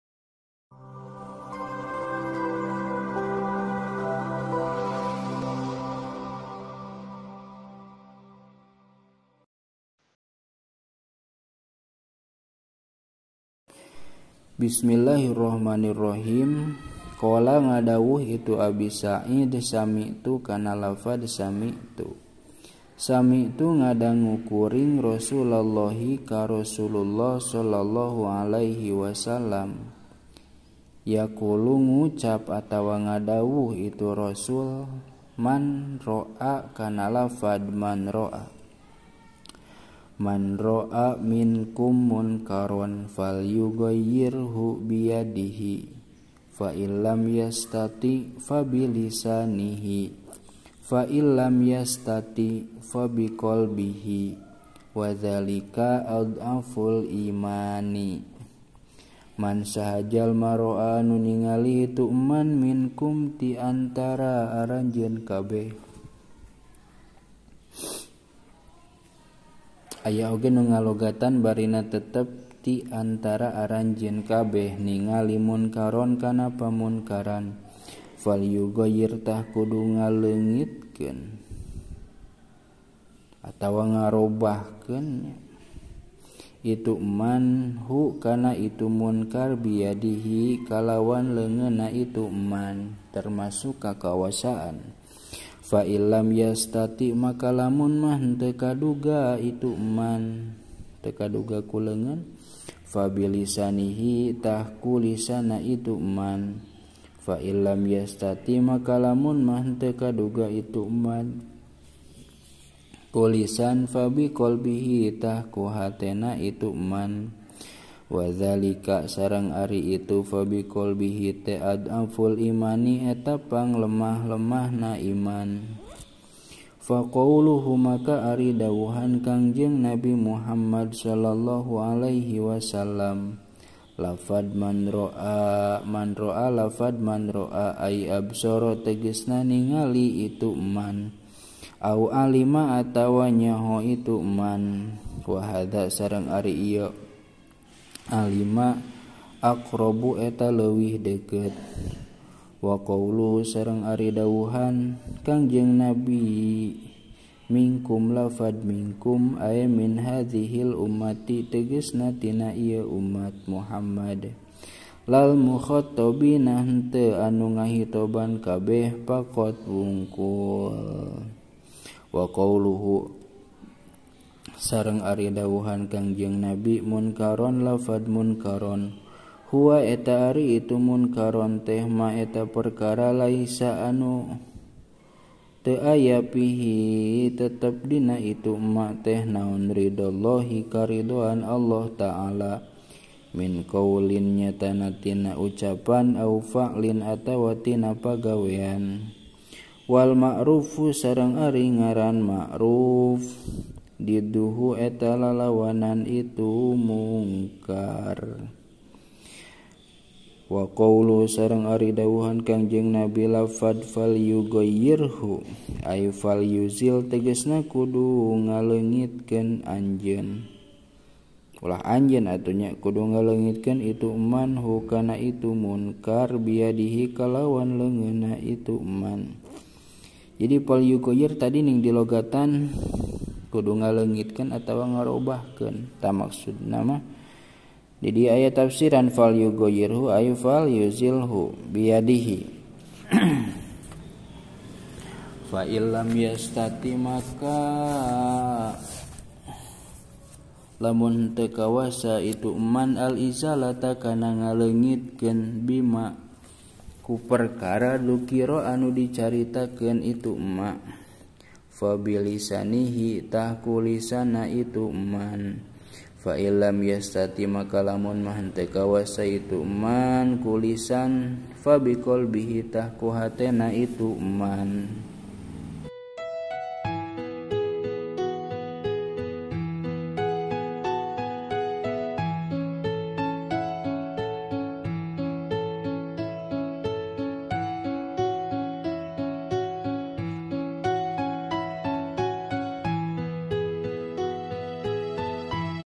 Logat Sunda Syarah Sulam Attaufiq #111 | Qouluhu : Qola Sami'tu... [Halaman : 16]
[Assalamu'alaikum...]

Dalam video ini berisi :
Logat sunda kitab Marqotu Shu'udittashdiq (Syarah Kitab Sulam Attaufiq)

Muallif Kitab Marqotu  Shu'udittashdiq :
Syekh Muhammad Nawawy
العالم الفاضل والورع الكامل الشيخ محمد نواوى

Muallif Kitab Sulam Attaufiq :
Syekh Abdulloh Bin Husen Bin Tohir Bin Muhammad Bin Hasyim Ba'alawy
العلامة الشيخ عبد الله بن الحسين بن طاهر بن محمد بن هاشم باعلوى

Note :
✓ Semoga bermanfa'at.

By.